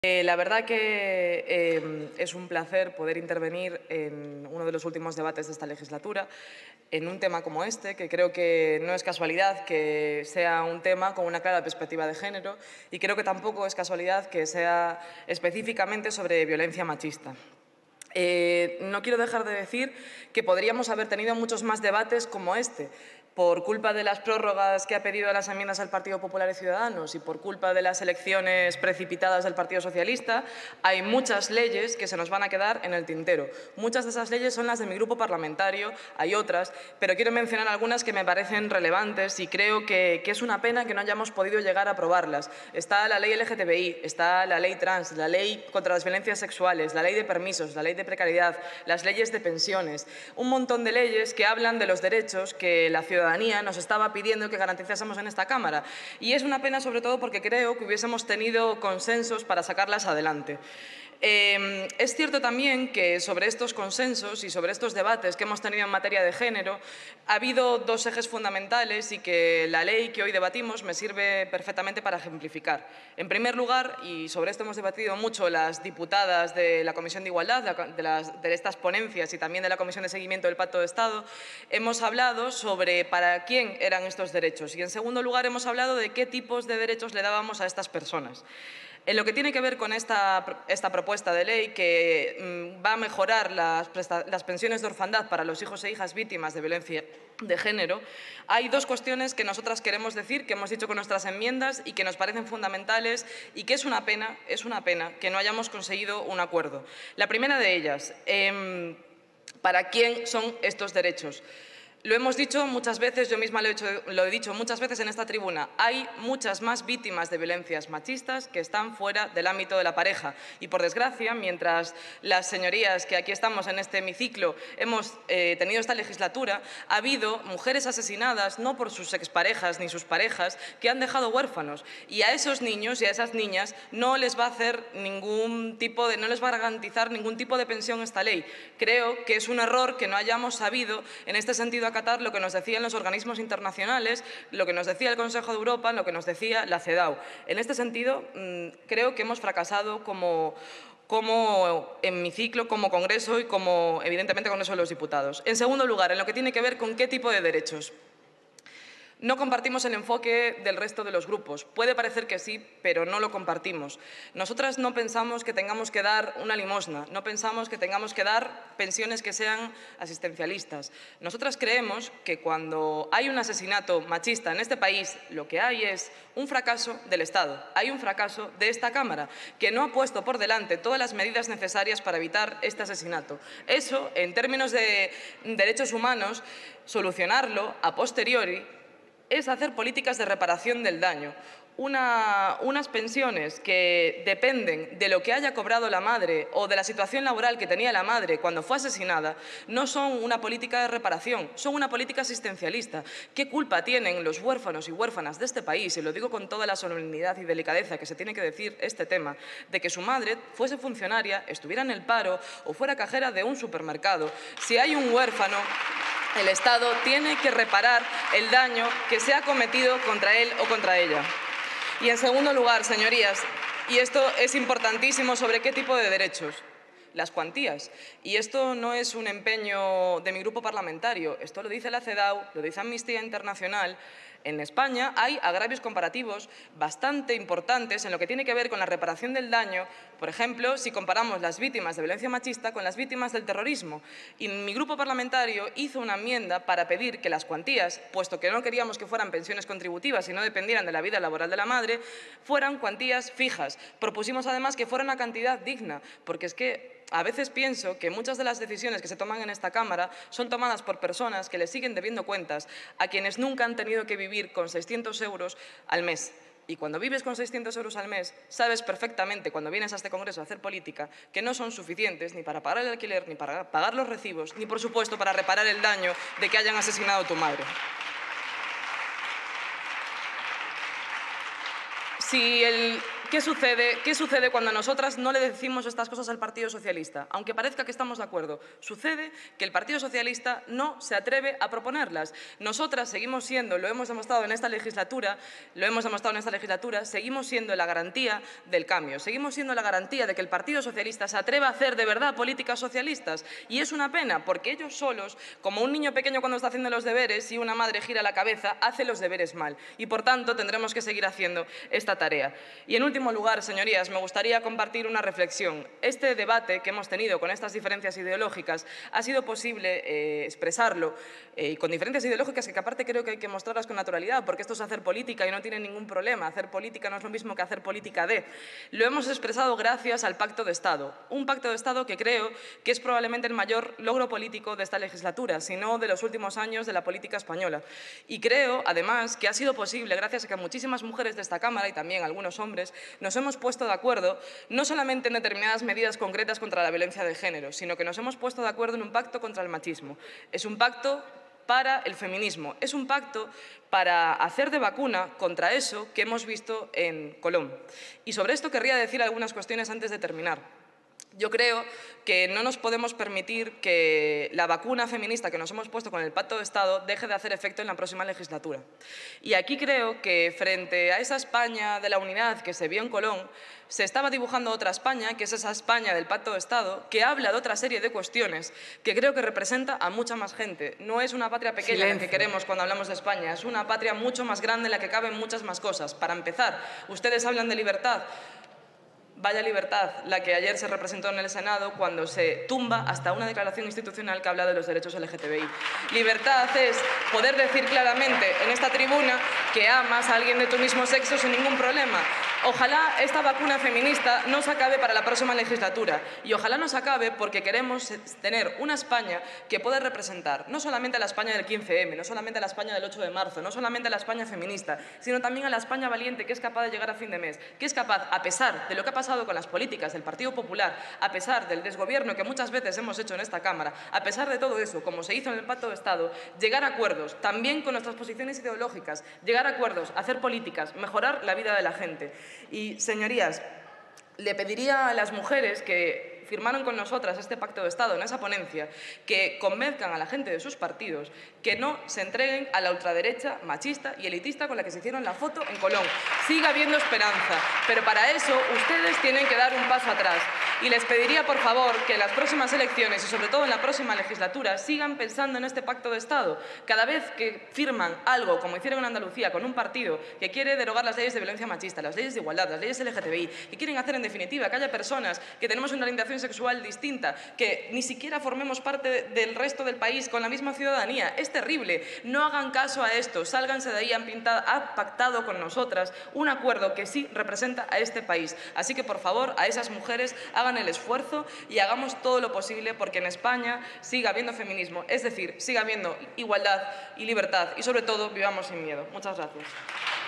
Angela Rodriguez: "Ha habido mujeres asesinadas que han dejado huérfanos, y no van a tener pensión".
Angela Rodriguez en el Pleno del Congreso de los Diputados el 21 de febrero de 2019.